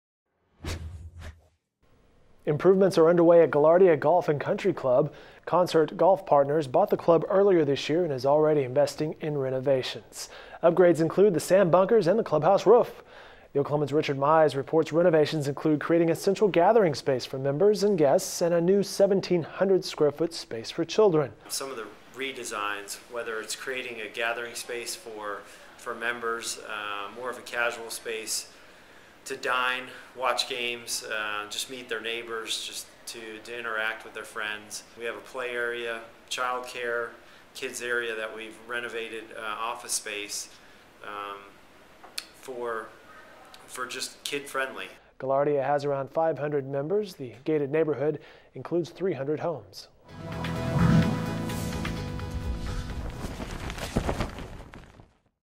New owners set new course for Gaillardia (2014-05-09)
Concert Golf Partners bought the troubled 15-year-old club with cash to spare, and they’re spending it on a new clubhouse roof and other improvements inside and out.